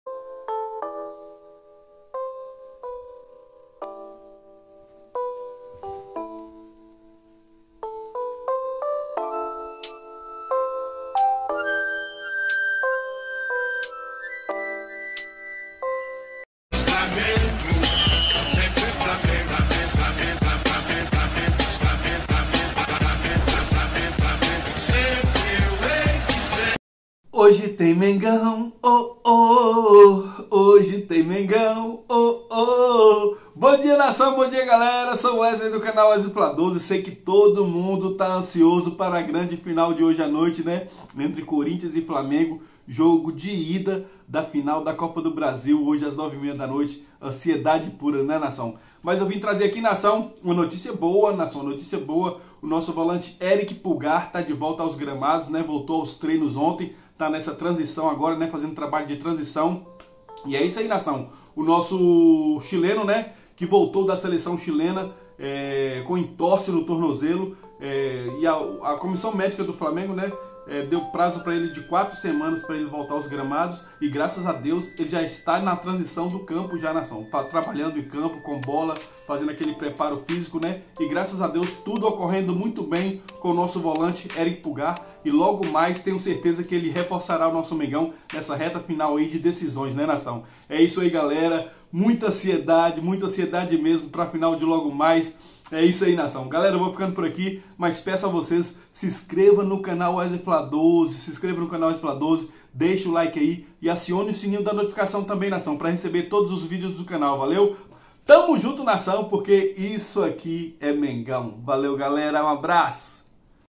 TREINO DO FLAMENGO ERICK PULGAR FAZ TRANSIÇÃO NO GRAMADO | ERICK PULGAR DE VOLTA AOS TREINOS NO FLA
COLABORE COM O CANAL WESLEY FLA 12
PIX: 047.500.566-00